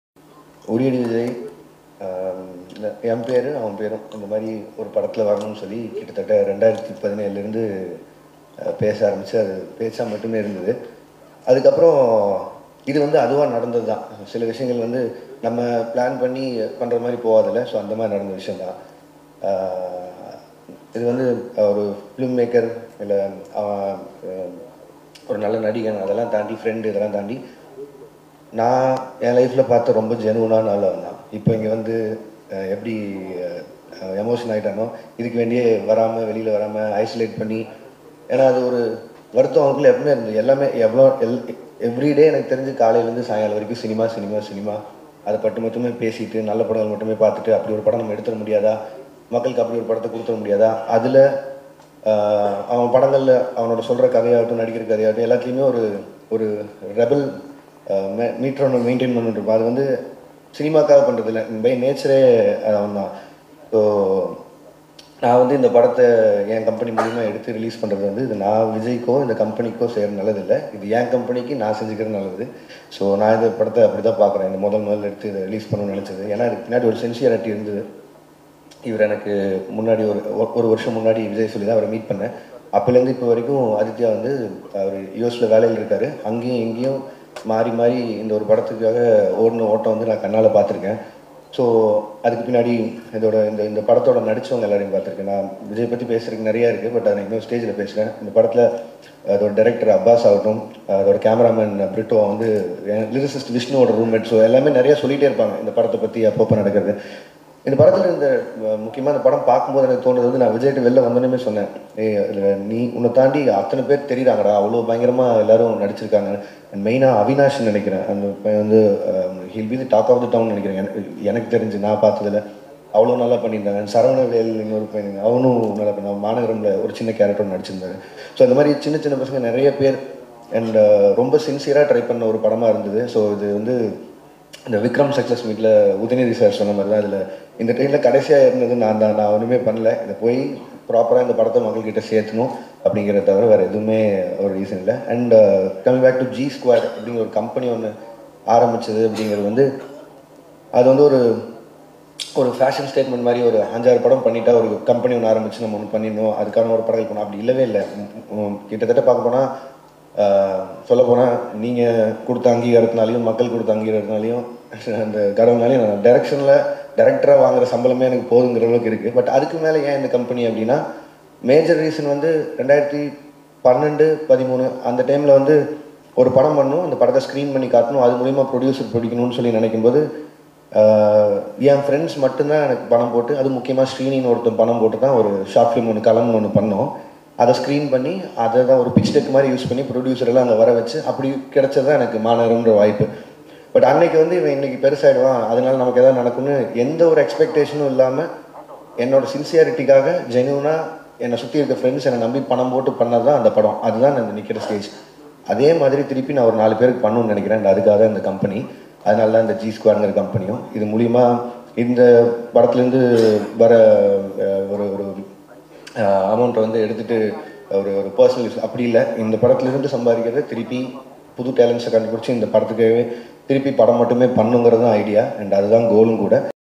GSquad Start பண்ணதுக்கு முக்கியமான காரணம் இதுதான்😎 - lokeshkanagaraj super speech
GSquad Start பண்ணதுக்கு முக்கியமான காரணம் இதுதான்😎 - LokeshKanagaraj Superb SPeech🔥